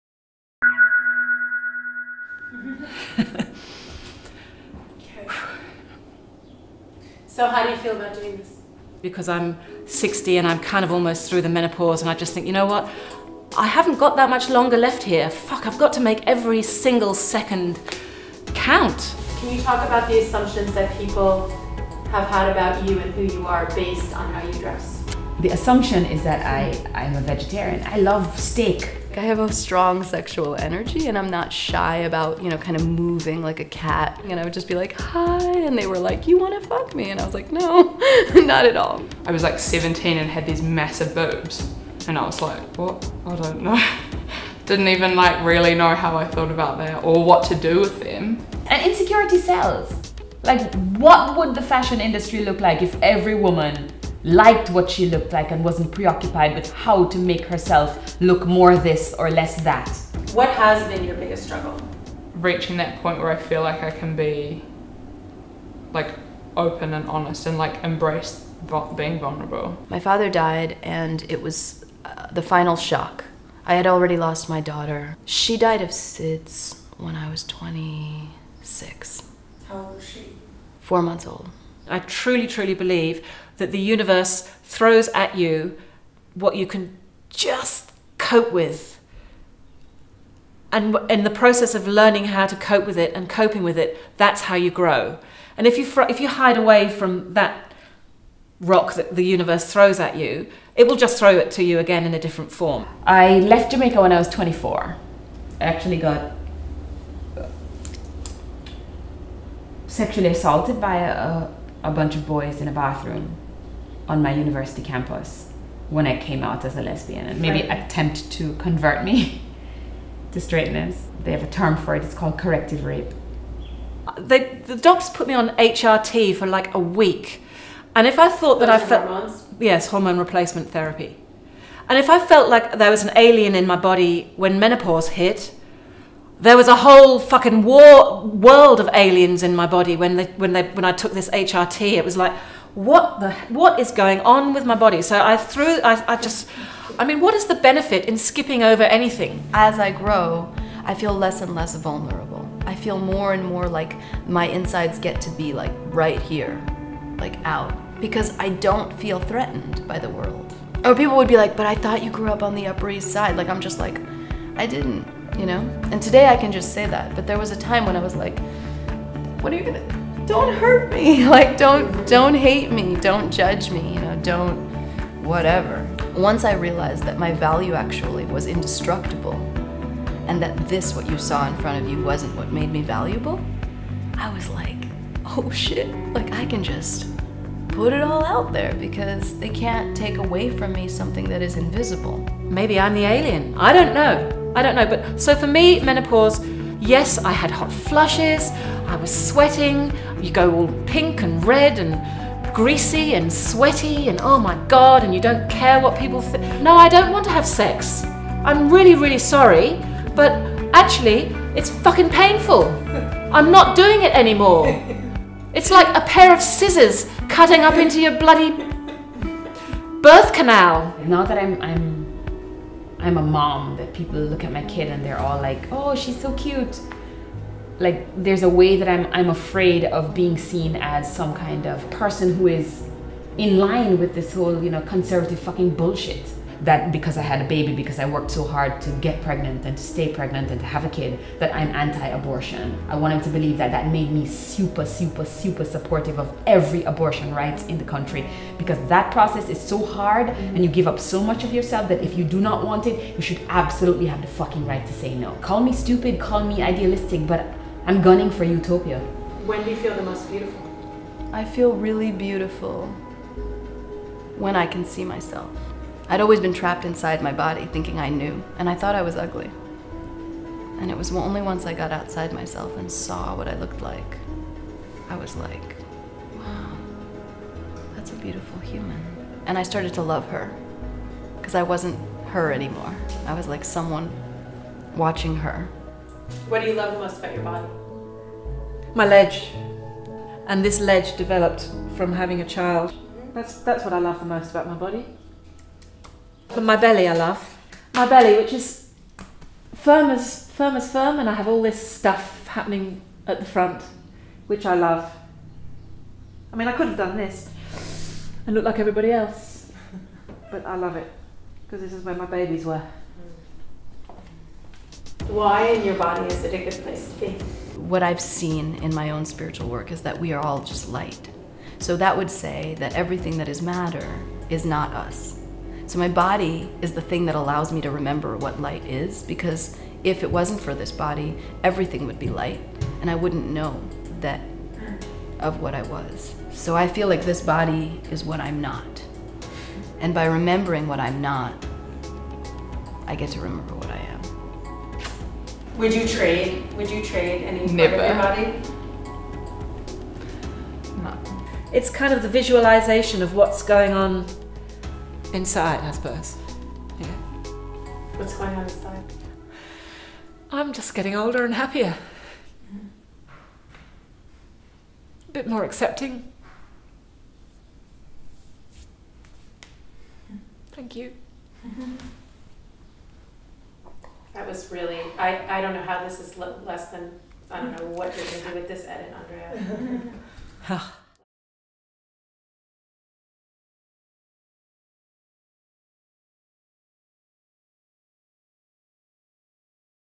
StyleLikeU x Olie Biologique Trailer
Starting Monday, each episode of the What's Underneath Project, will feature the journeys of four powerfully unique women, each of whom have worked with Olie Biologique to create their own body oil that embodies their individuality.

Each Oil is a special limited edition, with just 25 bottles available by each What’s Underneath superstar.

The videos launch on Monday.

The first oil is available Friday. Ten percent of the proceeds go towards our movement for self-acceptance.